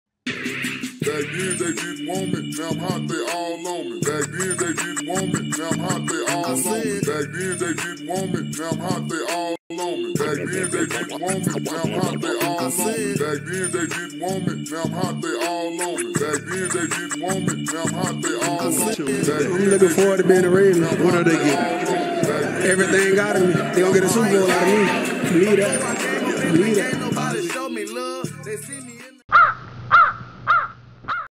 Lamar Jackson Rise To Fame Who Is Mike Jones?
Lamar Jackson Rise To Fame in the NFL and his first playoff win over the Titans last Sunday and Who is Mike Jones. Baltimore Play the Bills Tonight for a chance to play for the AFC crown.